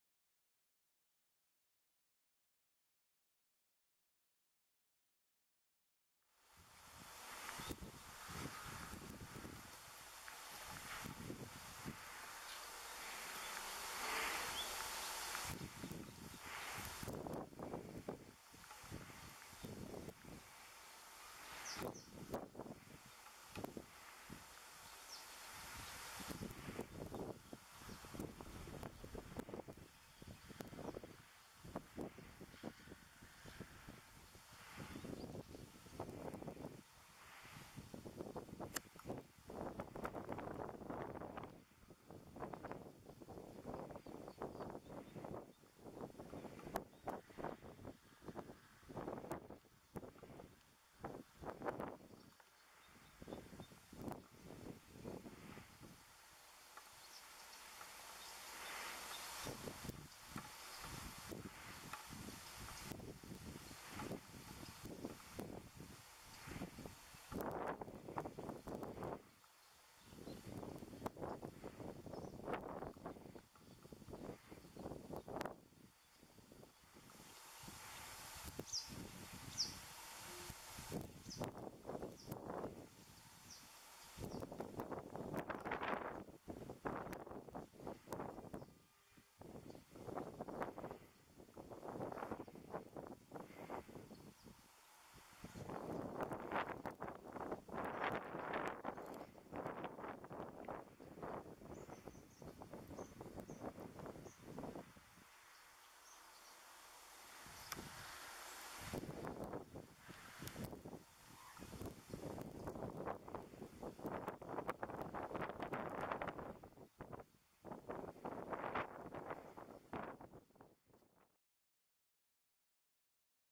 Yellow Wagtail (M. f. feldegg)
Also known as Black-headed Wagtail. Filmed at Zakaki Pool, Cyprus, on 23rd March 2015 with a Canon PowerShot SX50 HS.